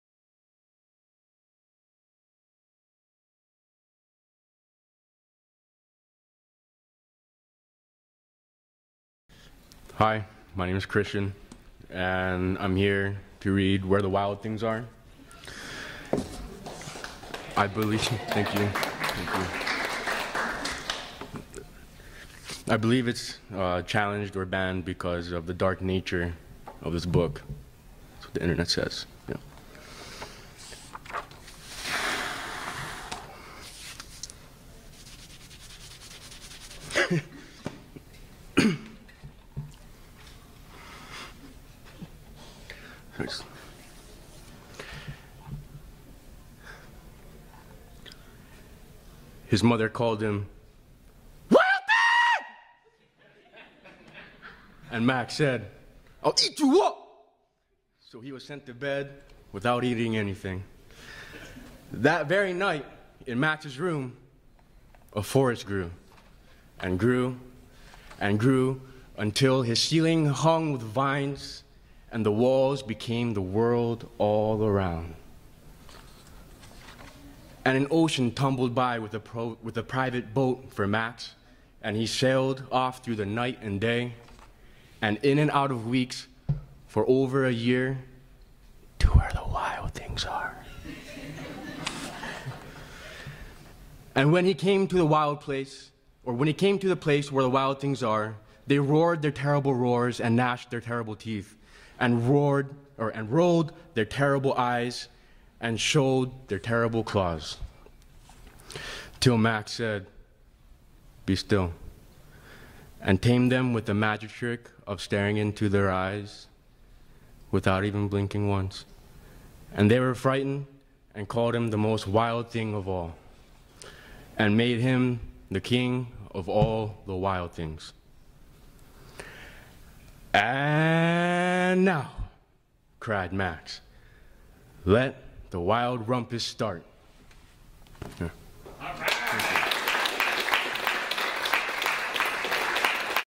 WCC BBW 2015 Virtual Read-Out: Where the Wild Things Are
The Windward Community College Library and Language Arts Department celebrated Banned Books Week by holding a read-aloud on September 30, 2015.